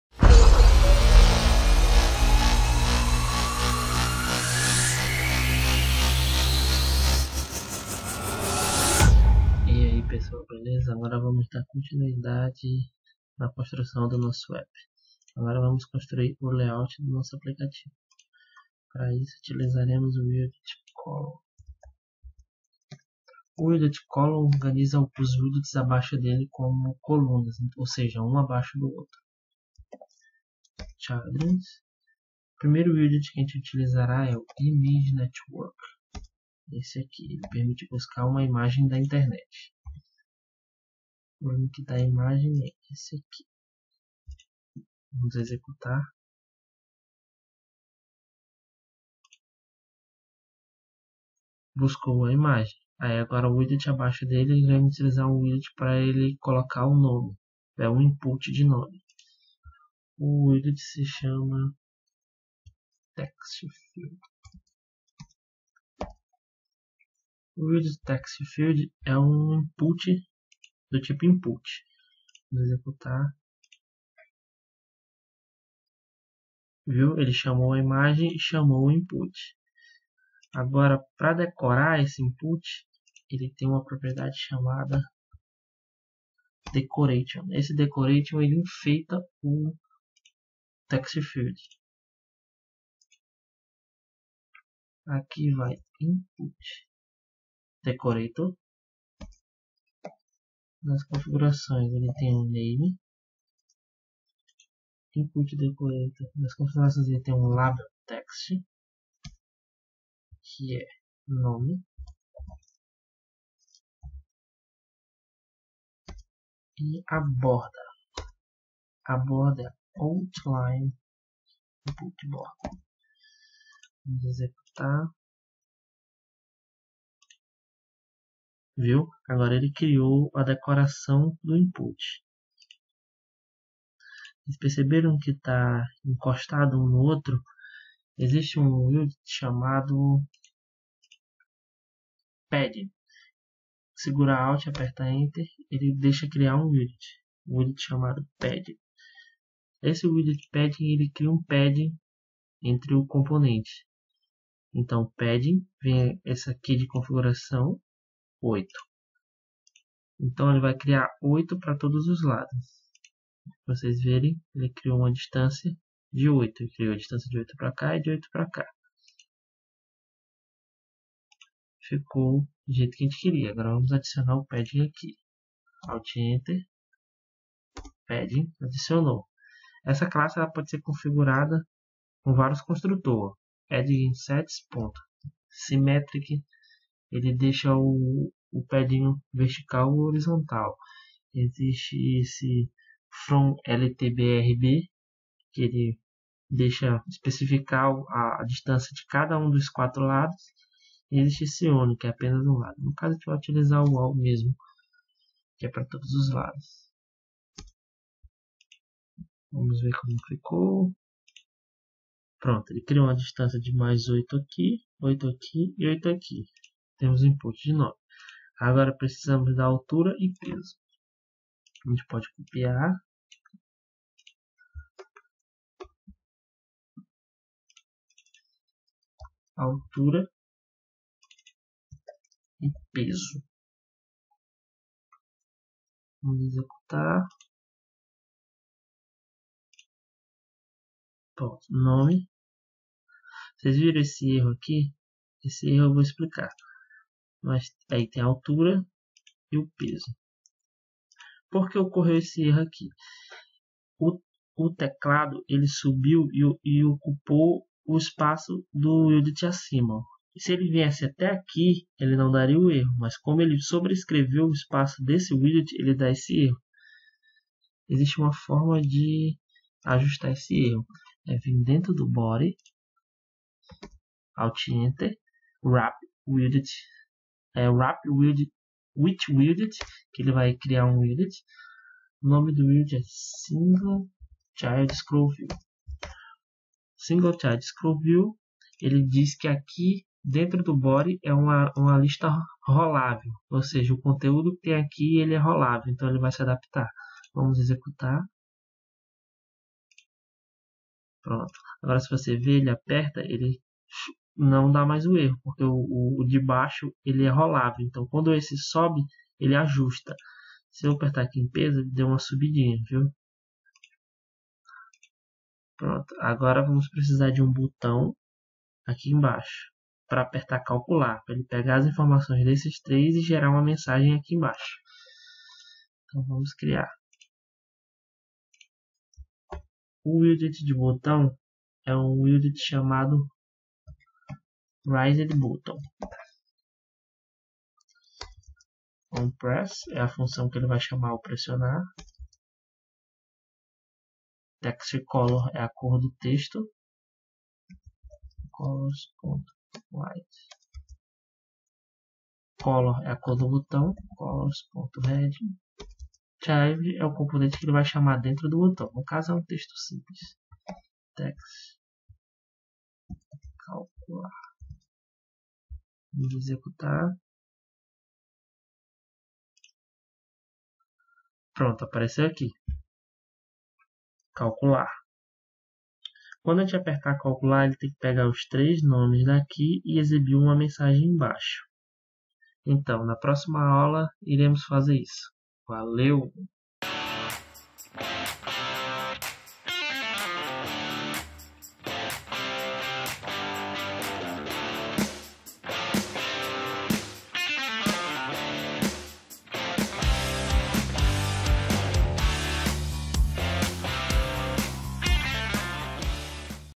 Flutter 04 - Módulo 1 - Finalizando a construção do layout
Nesse curso de Flutter você vai aprender na prática a como criar aplicativos. Durante o curso, você vai ver passo a passo como usar com Flutter e a linguagem Dart pra construção de diversos aplicativos.
Cada módulo mostra passo a passo como criar um aplicativo, aumentando a complexidade dos aplicativos criados a cada novo módulo.